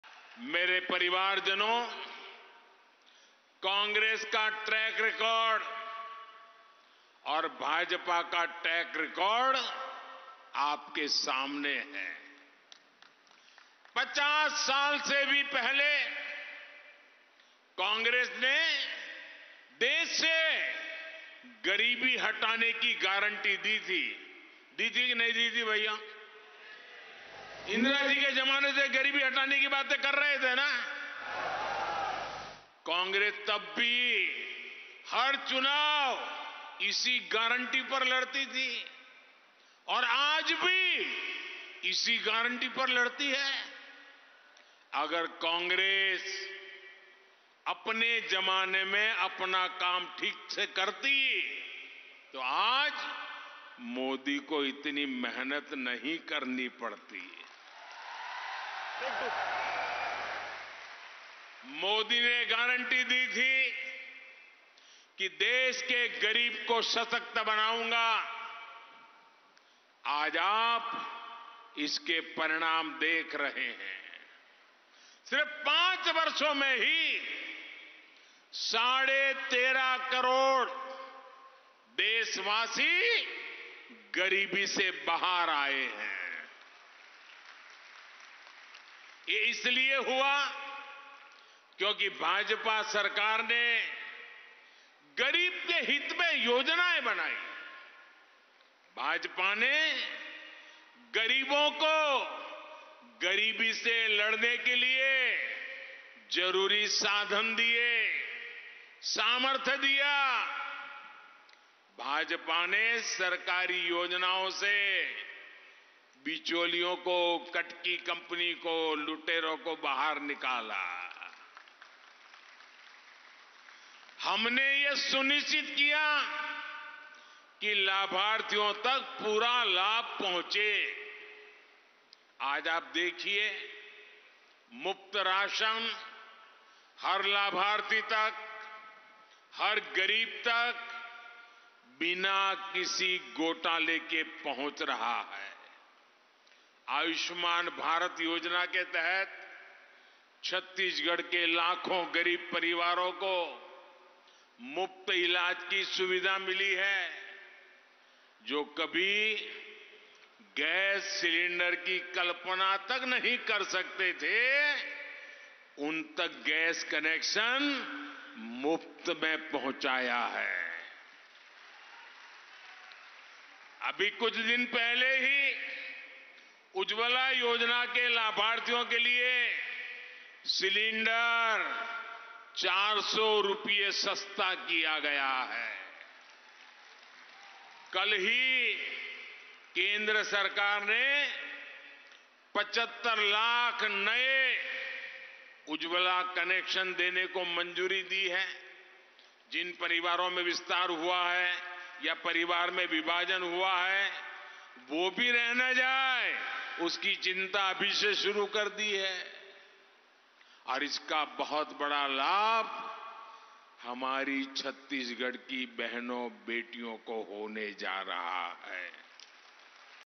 गारंटी के ट्रैक रिकॉर्ड पर पीएम मोदी का कोई सानी नहीं, आंकड़े देते हैं गवाही
After inaugurating the various rail infra projects, Prime Minister Narendra Modi addressed a public rally in Raigarh, Chattisgarh.

प्रधानमंत्री नरेन्द्र मोदी ने विभिन्न रेल इंफ्रास्ट्रक्चर प्रोजेक्ट्स का उद्घाटन करने के बाद छत्तीसगढ़ के रायगढ़ में एक सार्वजनिक रैली को संबोधित किया।

PM Narendra Modi Live,
Modi Live,
Modi Speech,
pm modi speech today,
Narendra Modi,